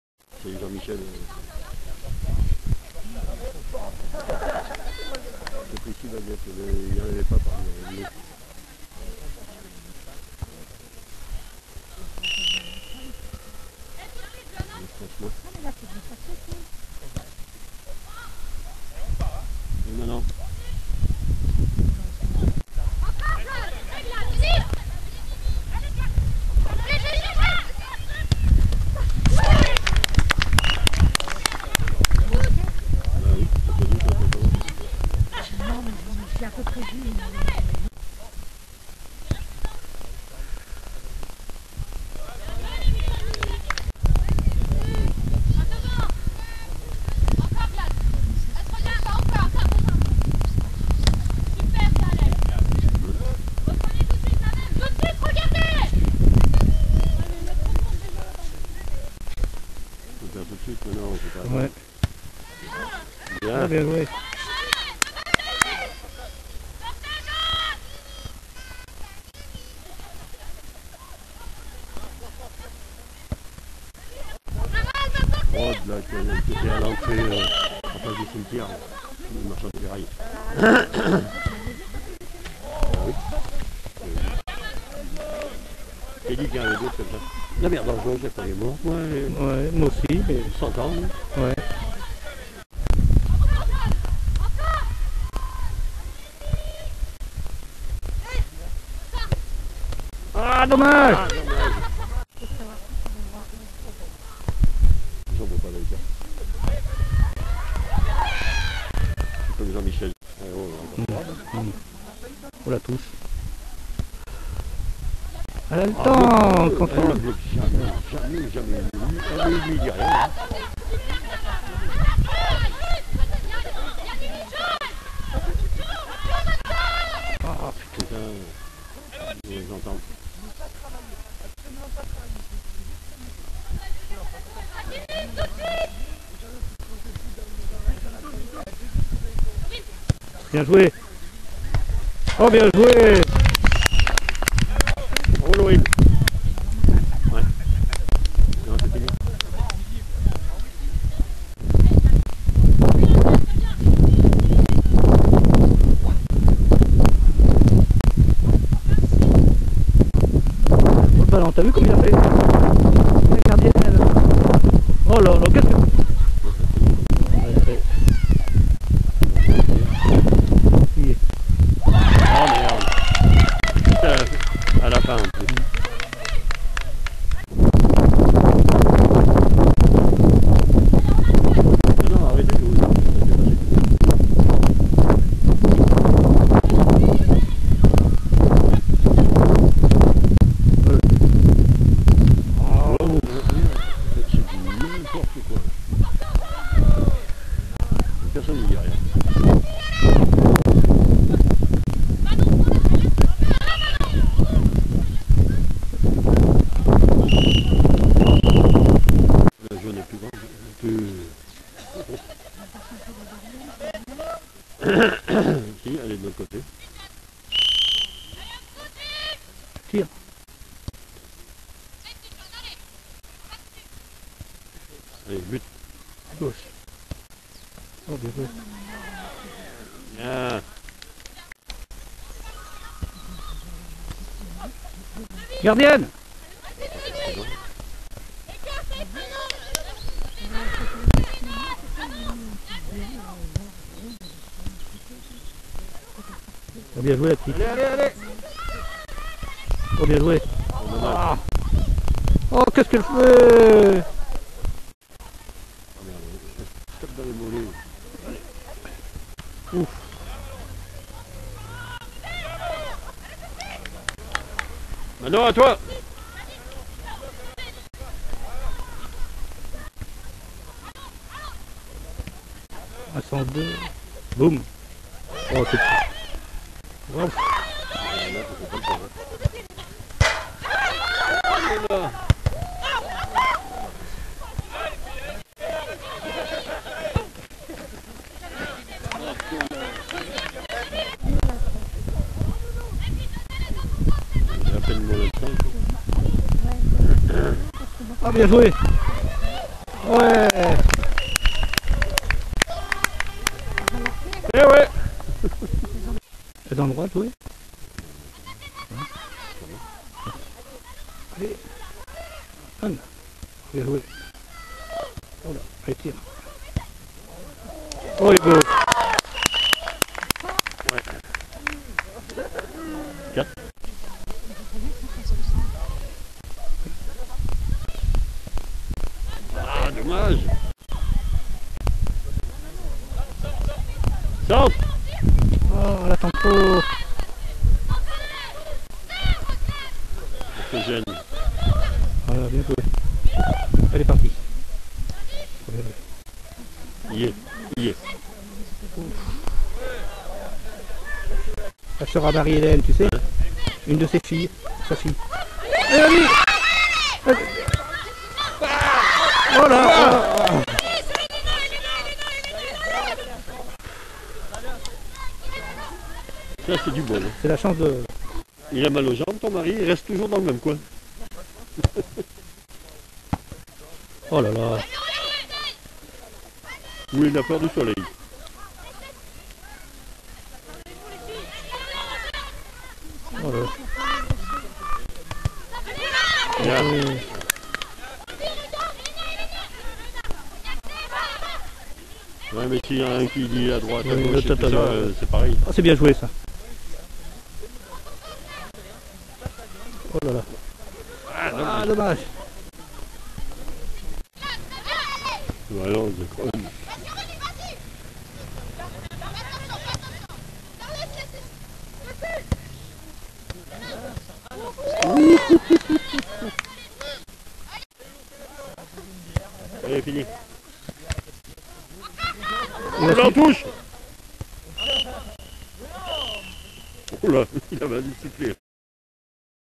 ENT SUD EST / U S FELLETIN - ENT EVAUX BUDELIERE / ES MAINSAT / SANNAT
Féminines 1 ère Division ( 2020 / 2021 )
Ent Sud Est / US Felletin 4 - 1 Ent Evaux Budelière / ES Mainsat / Sannat